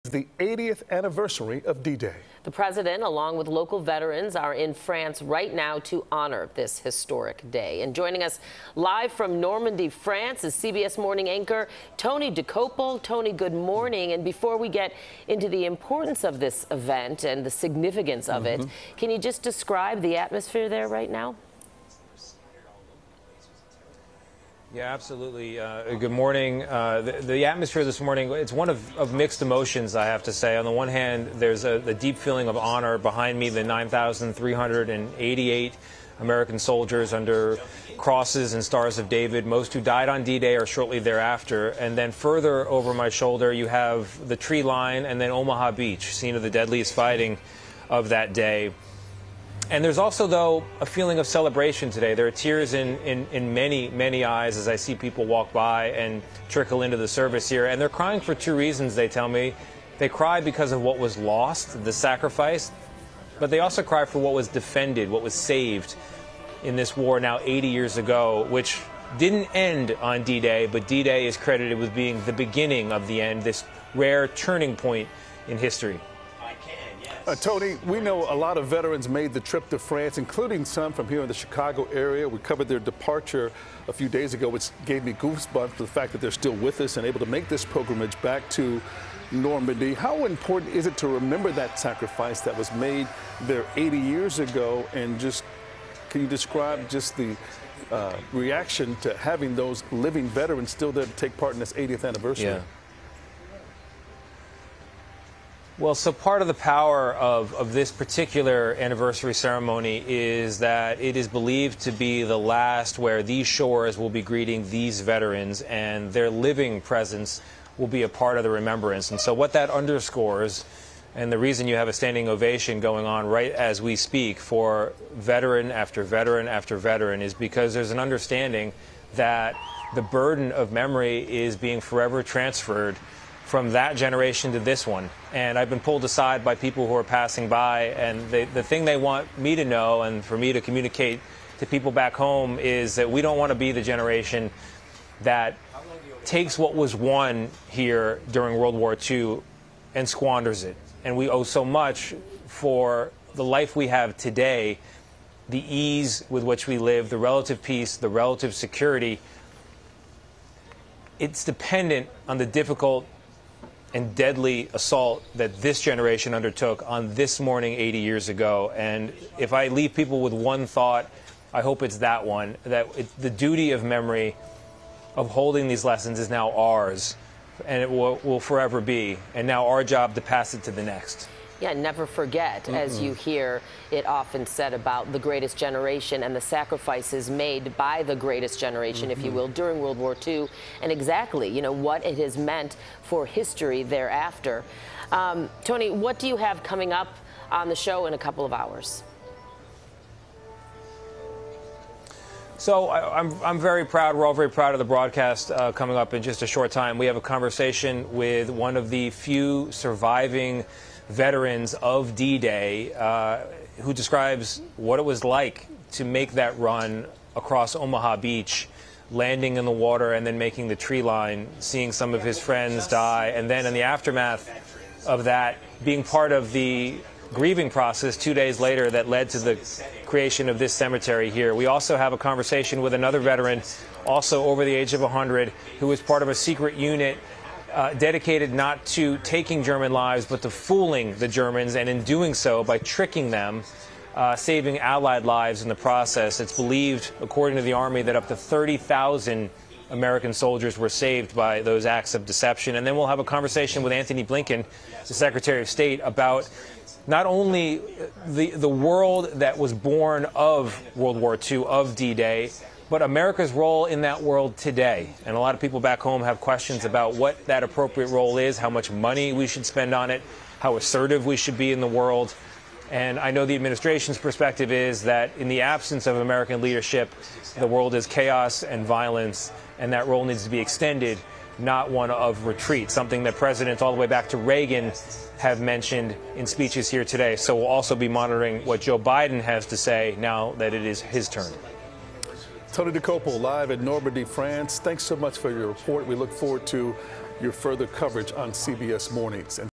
Biden, veterans honoring 80th anniversary of D-Day in Normandy, France
The president and veterans, some from the the Chicago area, are in France to honor the historic day.